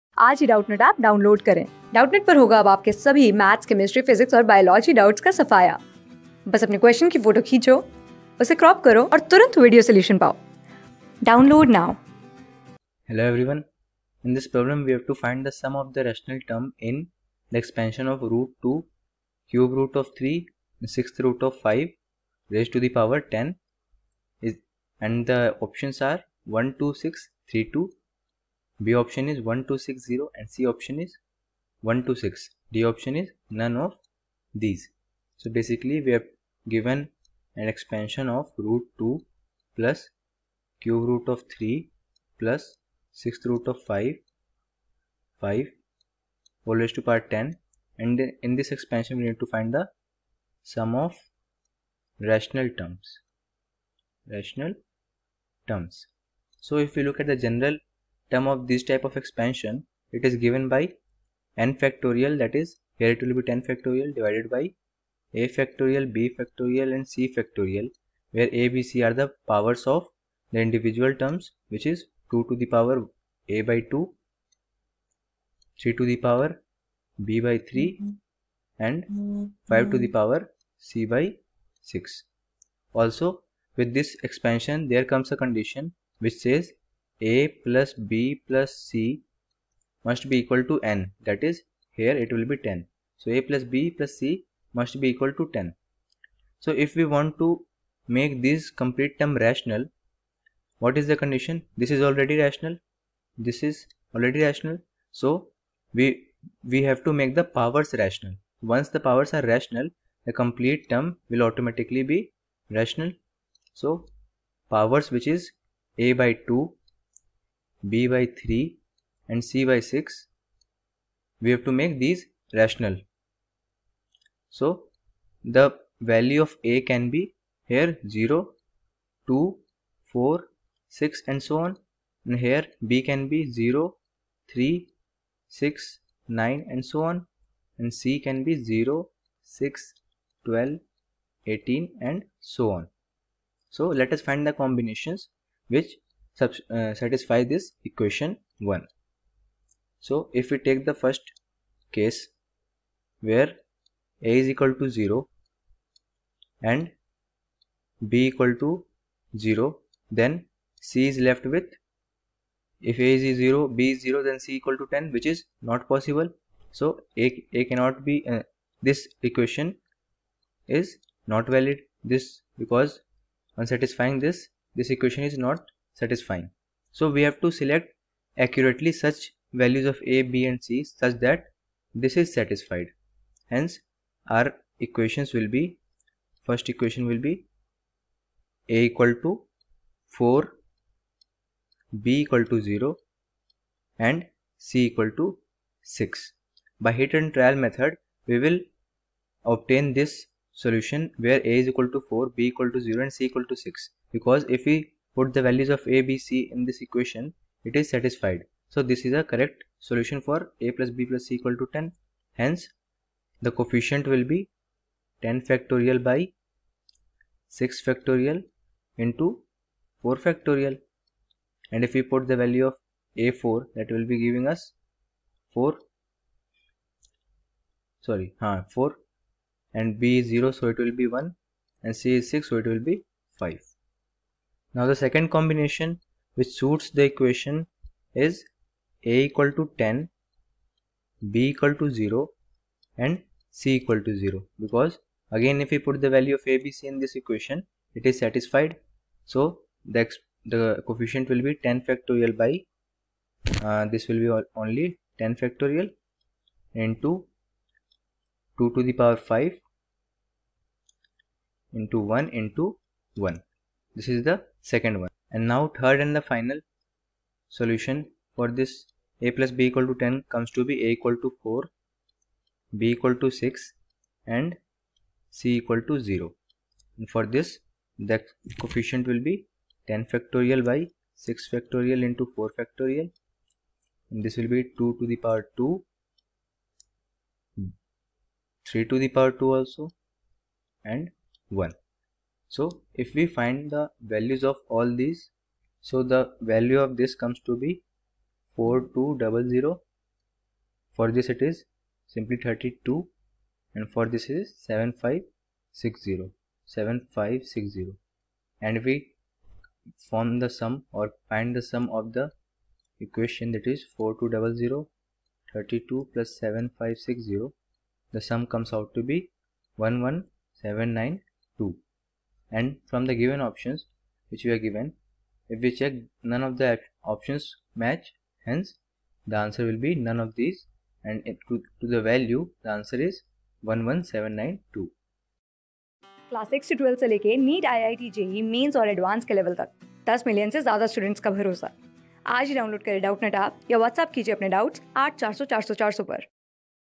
The sum of rational term in (sqrt(2)+root3 3 + root6 5)^(10)\nis equal to\n12632\nb. 1260\nc. 12...
The sum of rational term in (sqrt(2)+root3 3 + root6 5)^(10)\nis equal to\n12632\nb. 1260\nc. 126\nd. none of these
Class: 11
Subject: MATHS
Chapter: BINOMIAL THEOREM
Board:IIT JEE

👉 Have Any Query? Ask Us.